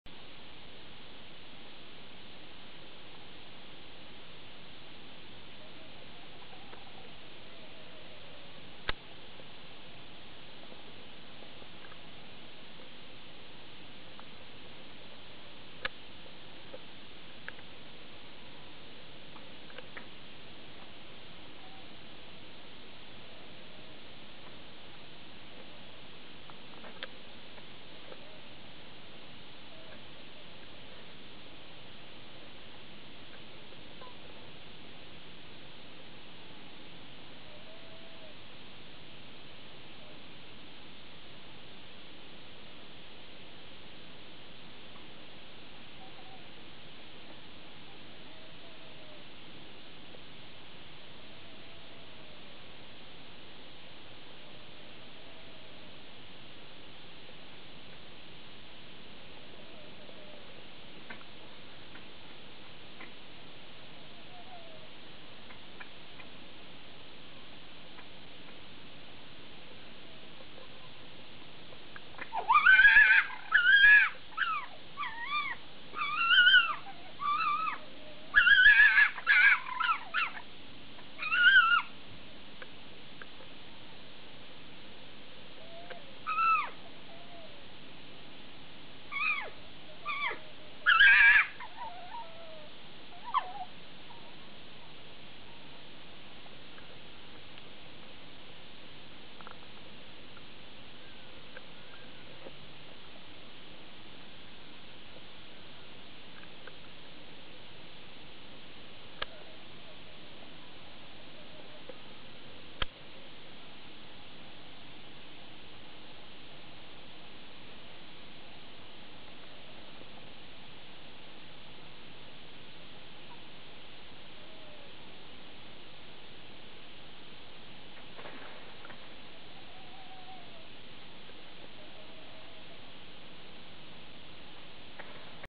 Foxes late at night in next door's garden.
I was being kept awake by these foxes at 2am on 28th June 2009 and was very lucky, watch the video to the end and you'll not only hear them but see one of them look straight up at the camera. :¬)